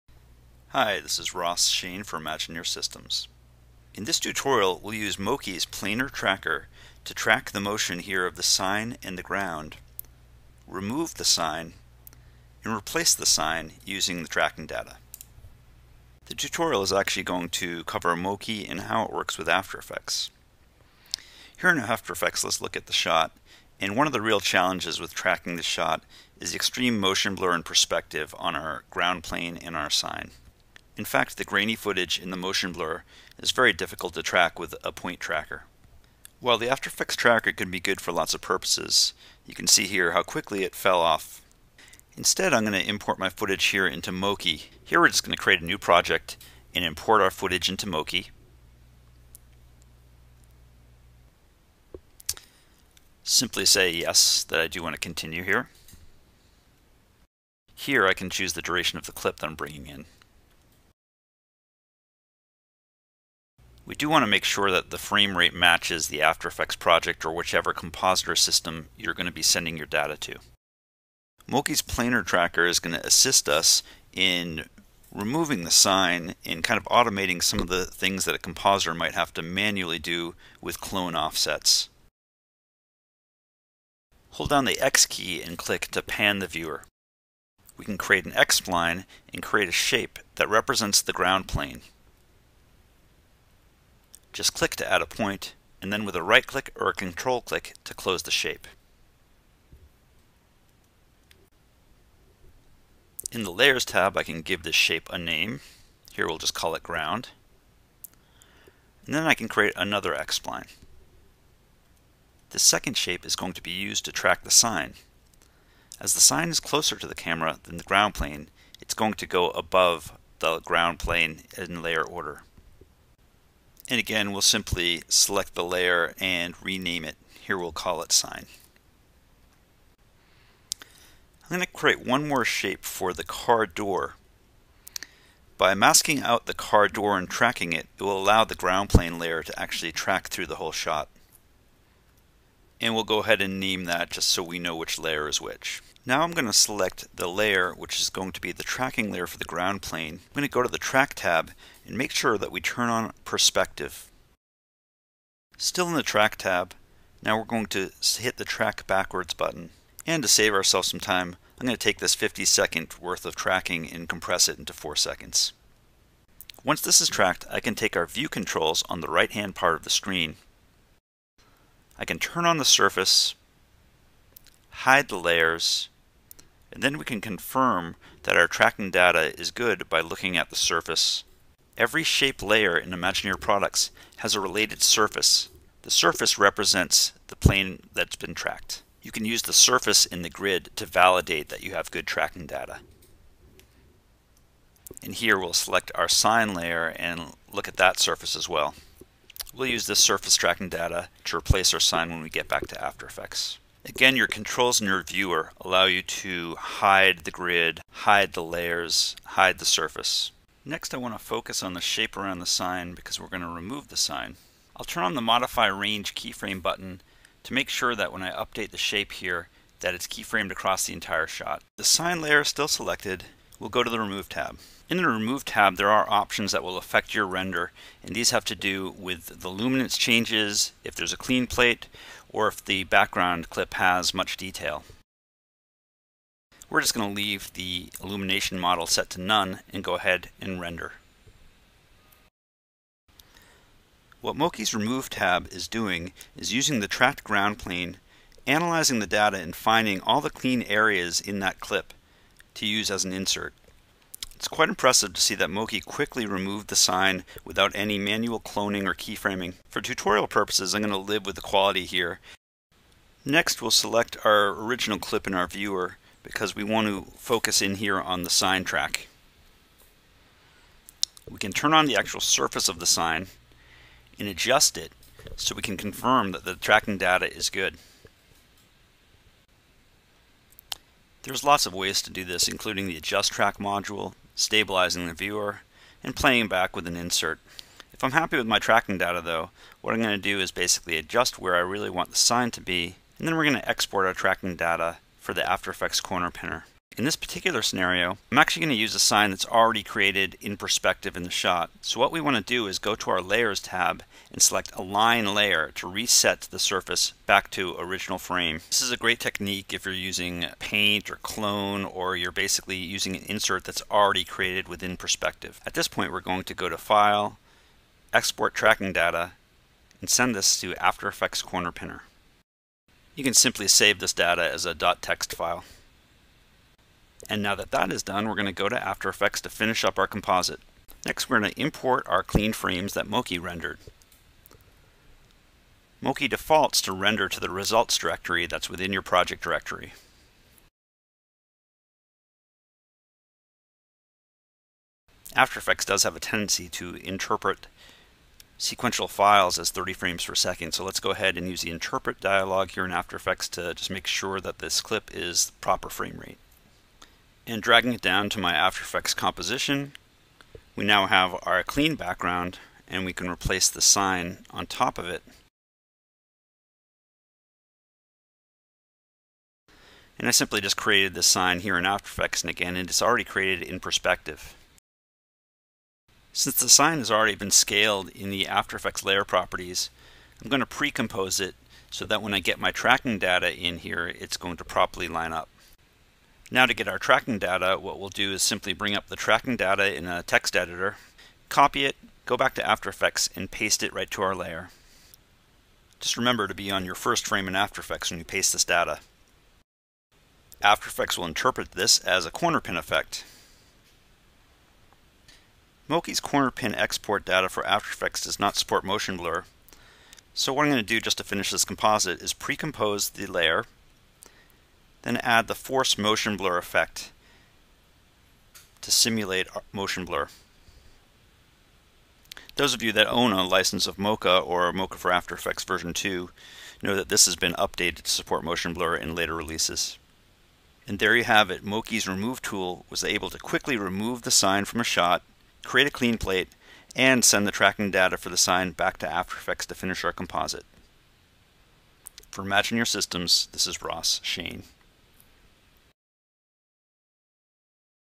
Tutorial: Mokey Sign Removal
Ross Shain of Imagineer Systems walks you through a quick removal job in mokey, then sends track data to After Effects to finish to composite.